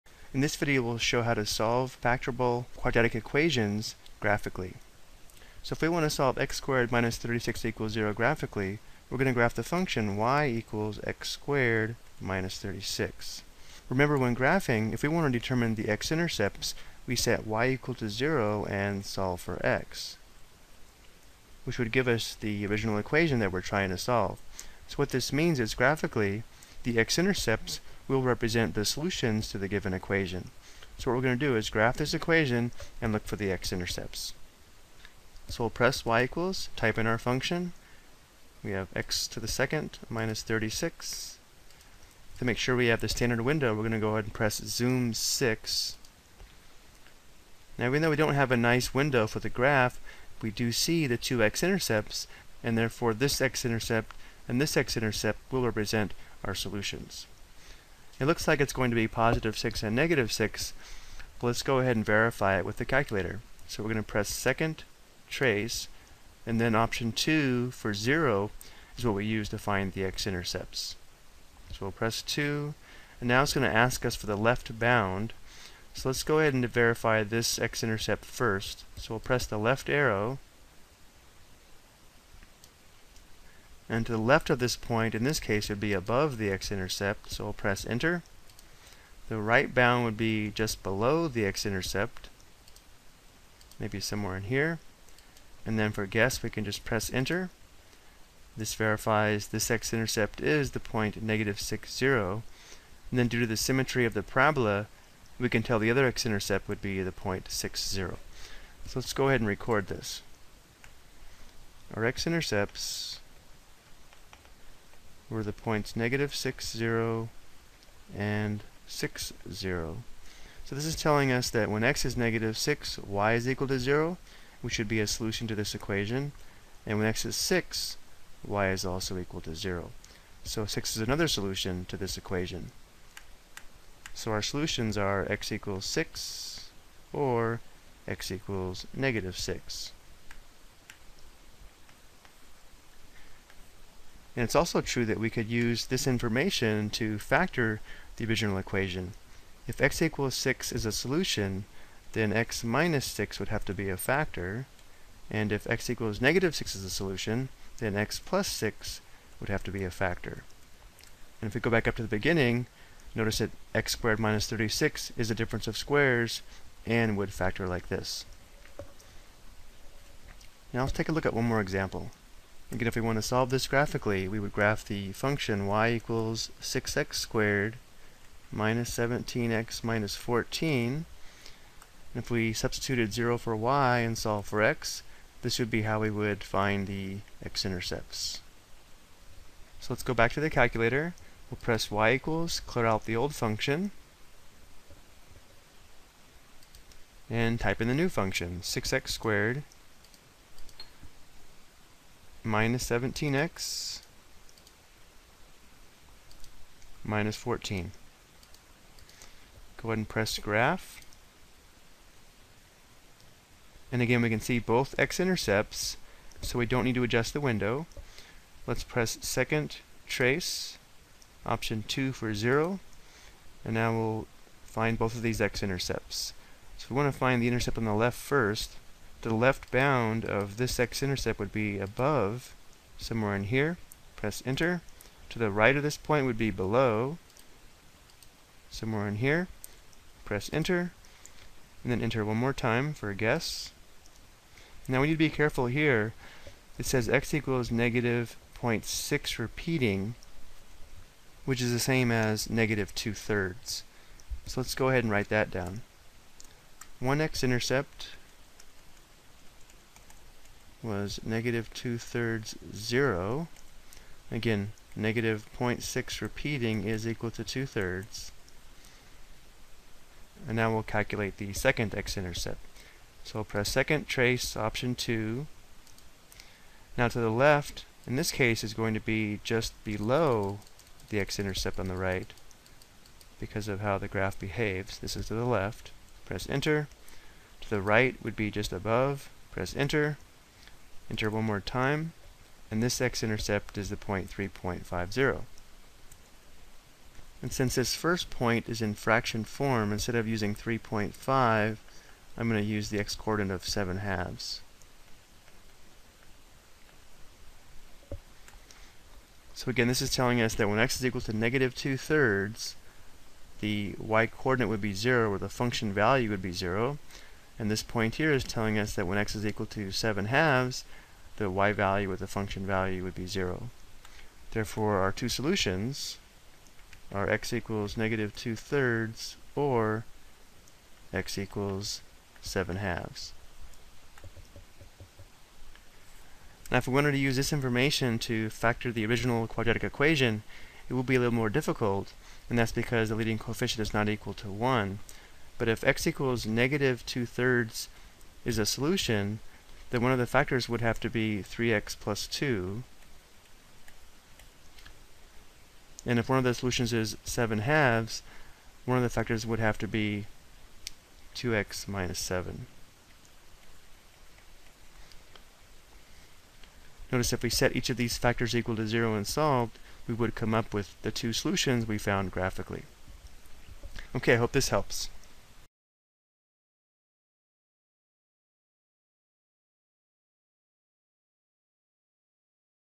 Ex 2: Solve a Quadratic Equation Graphically on Calculator
This video explains how to solve a factorable quadratic equation graphically.